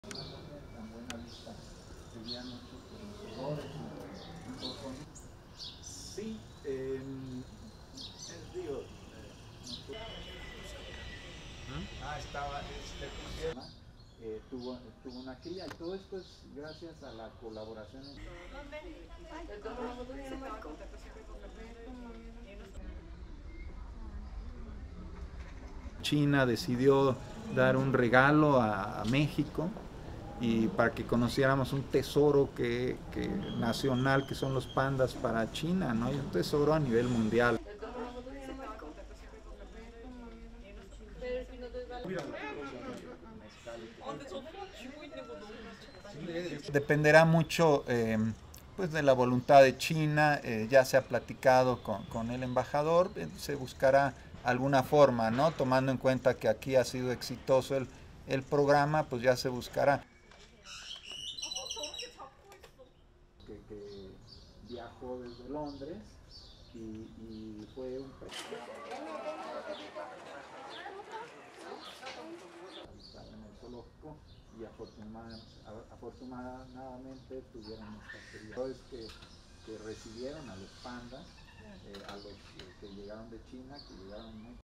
The last giant panda in Latin America
(17 Nov 2022) Xin Xin, the last giant panda in Latin America, sat inside her lush enclosure on a recent morning munching bamboo, unaware of the geopolitical forces that could continue or end nearly a half-century of delighted visitors in Mexico's capital. (Nov 17) (AP video: Gerardo Carrillo)